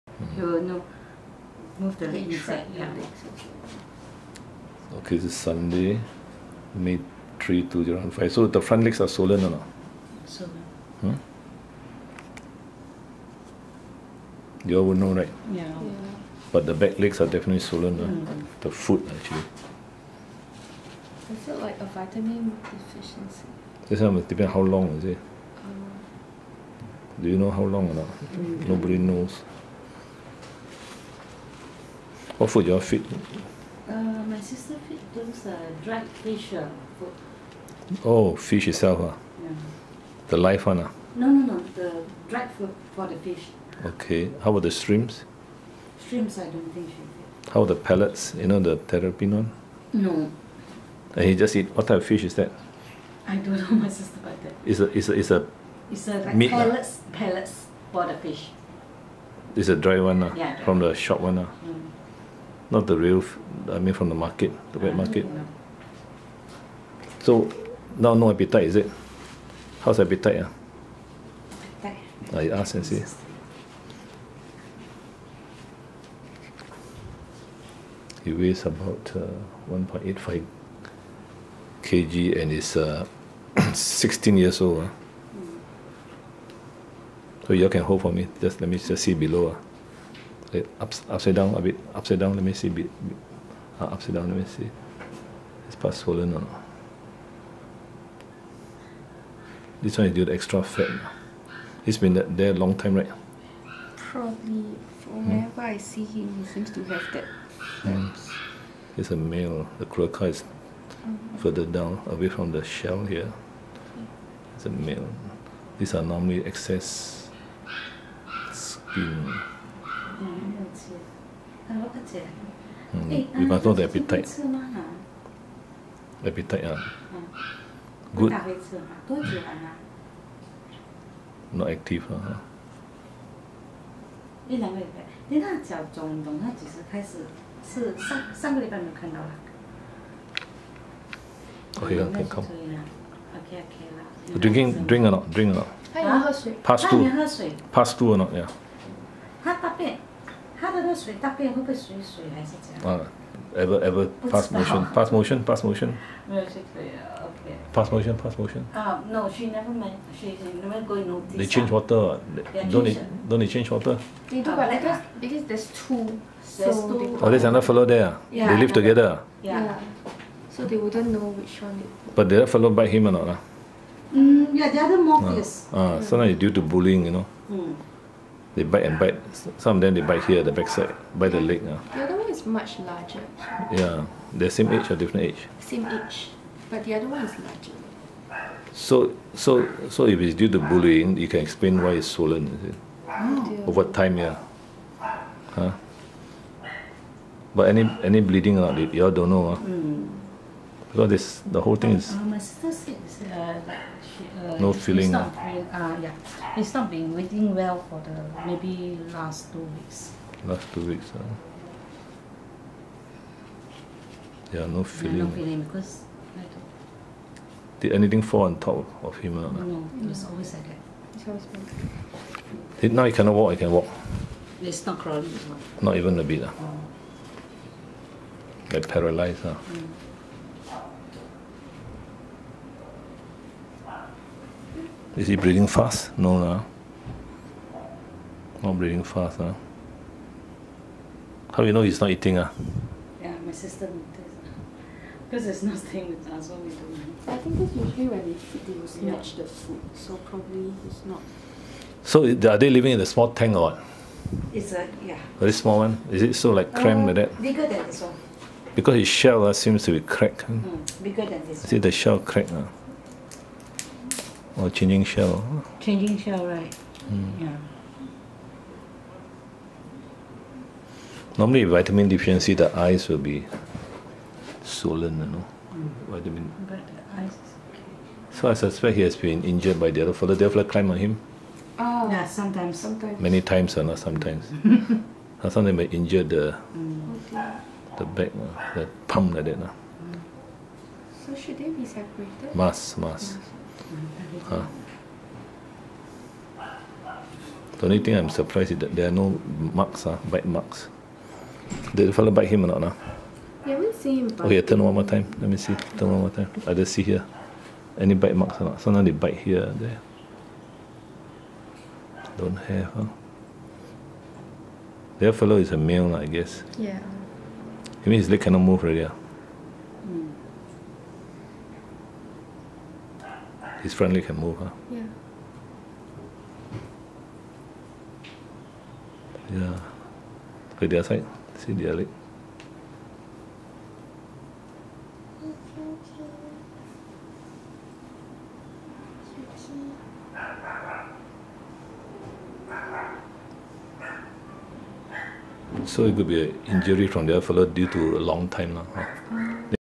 A 16-year-old terrapin is paralysed in his hind limbs.
Cracked upper shell is a clue. This terapin had been climbed over by the other terrapin over the years. Now she is paralysed in the hind legs. Trauma to the spine is likely the cause.

The other terrapin, housed in the same tank, has no cracked shell. To follow up after a course of painkillers and antibiotics.